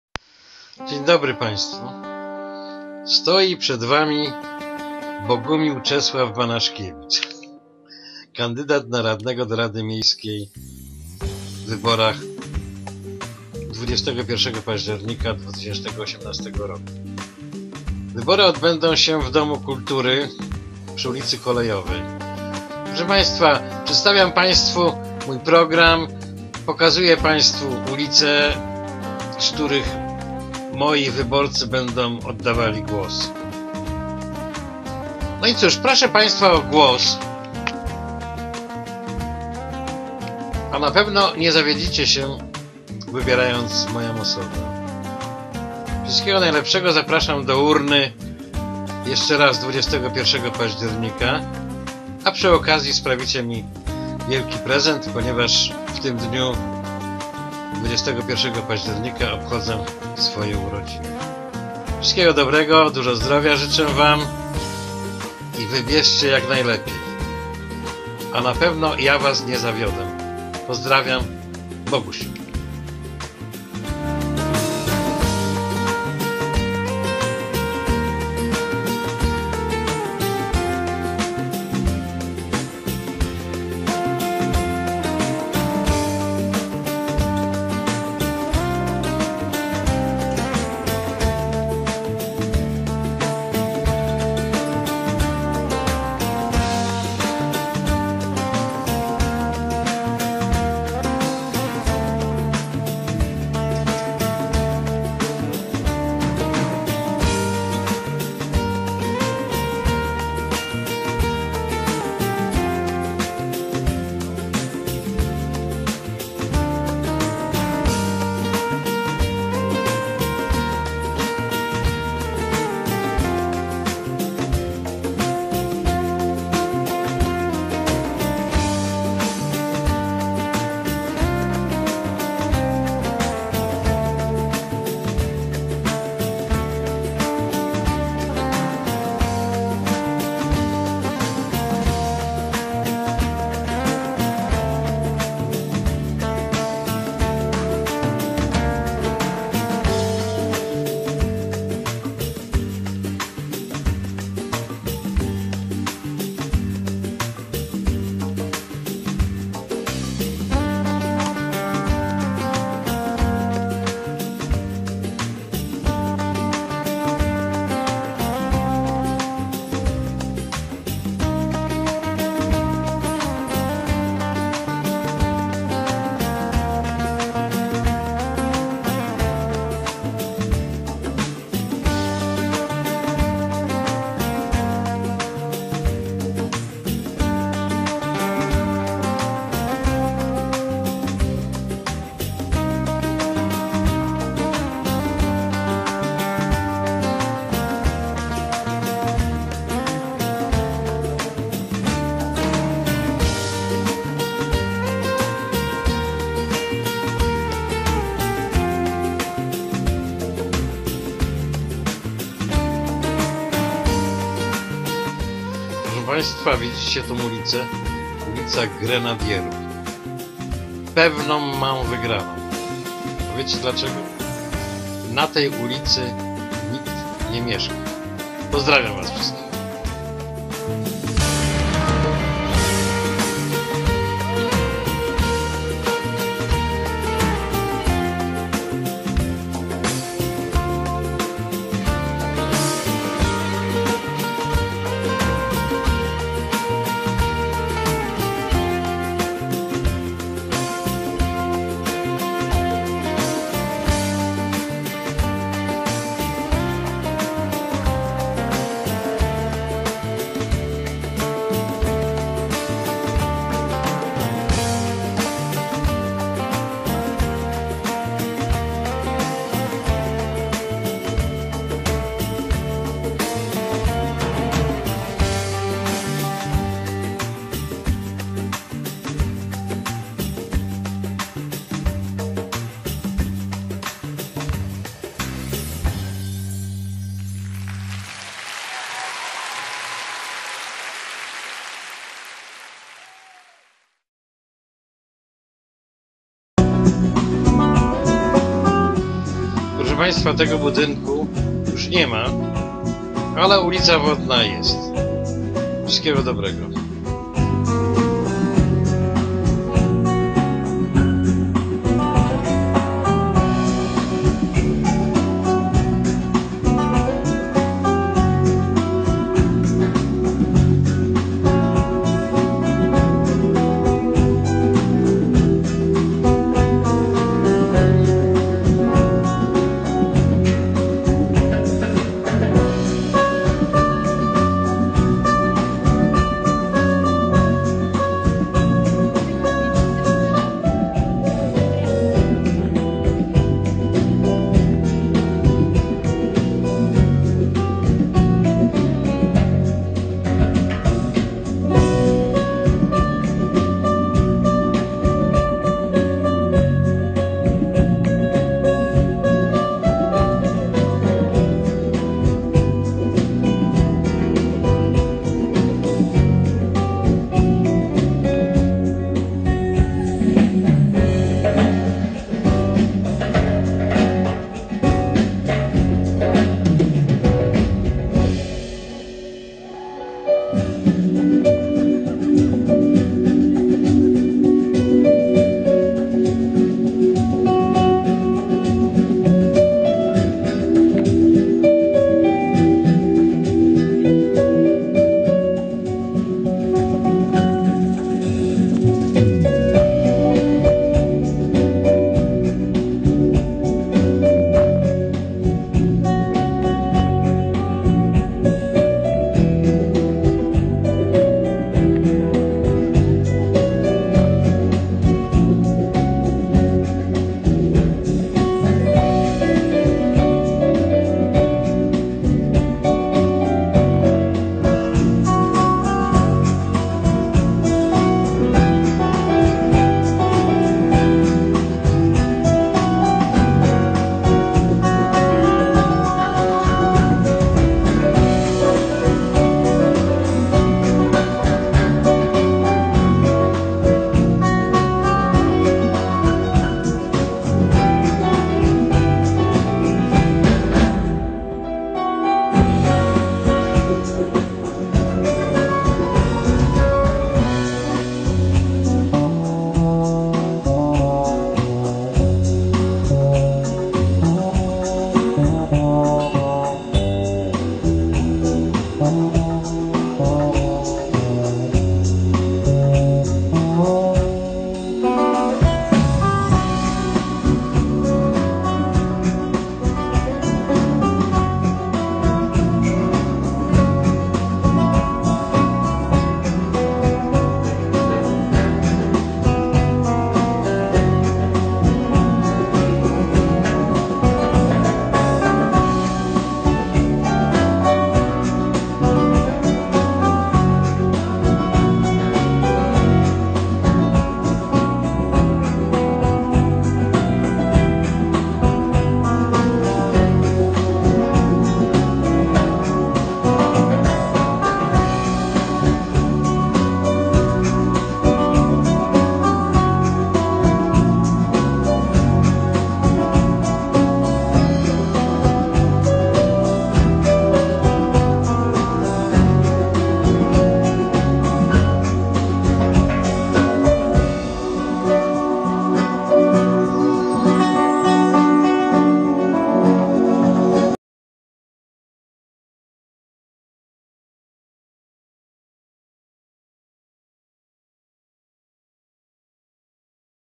Film - Mój klip wyborczy 13 października 2018 roku
Wybory samorządowe 2018 r w Brzegu Dolnym 21 października .W związku z tym ,że ubiegam się o mandat radnego do Rady Miejskiej w Brzegu Dolnym,postanowiłem zrobić film.A ponieważ nie stać mnie na bilbordy,banery,czy wielgachne plakaty,promuję siebie właśnie w ten sposób.